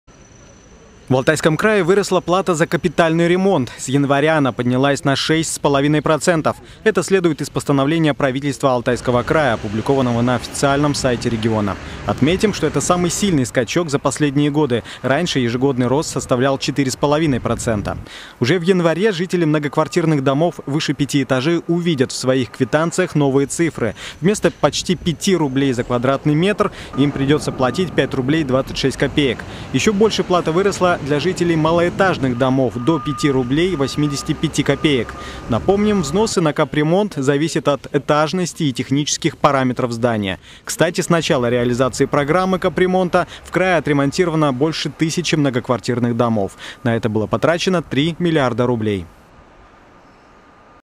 В Алтайском крае выросла плата за капитальный ремонт
С января она поднялась на шесть с половиной процентов.  Это следует из постановления правительства Алтайского края, опубликованного на официальном сайте региона.